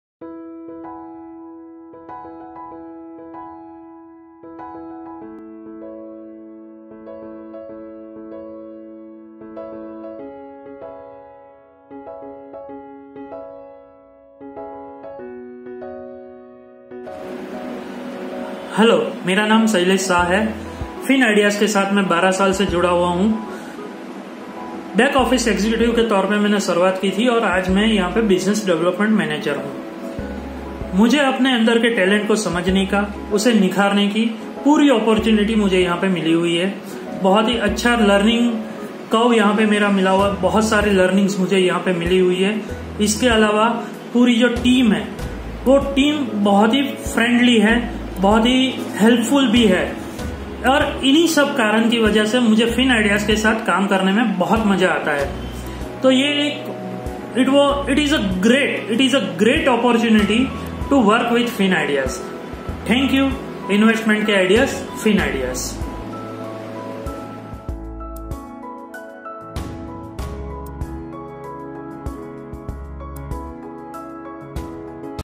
Happy Employees = Happy Company😊 Mr. Shailesh Shah has something to say about FinIdeas!
Customer Satisfaction starts with Employee Satisfaction. If they are happy at work, no one can stop an organization to be successful!

Have a look at what our Business Development Manager Mr. Shailesh Shah wants to say!

We value our employees as much as our clients. Mr. Shailesh we value your trust in us and are happy that you feel at home with FinIdeas family 🤗🤝